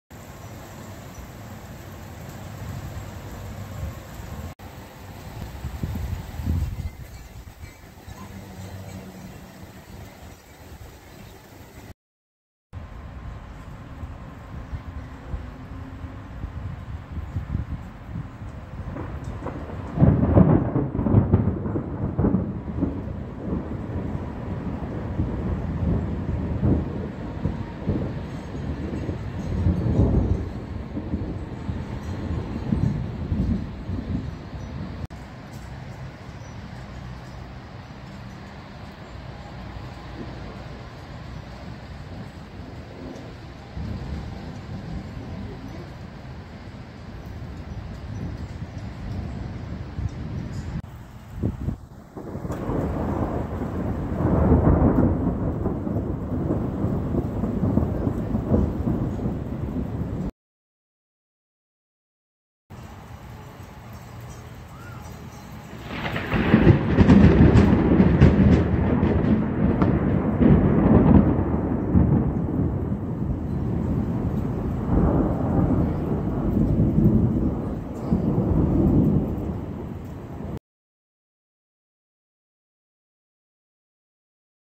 3/27/21 Severe Thunderstorms Roll Through St. Louis
A line of severe thunderstorms associated with a cold front are rolling east-southeast through the Saint Louis Metro. Threats include heavy rain, lightning, damaging winds and hail. A double-digit drop in temperatures will follow.

Free Space: free-space.social - Red Weatherman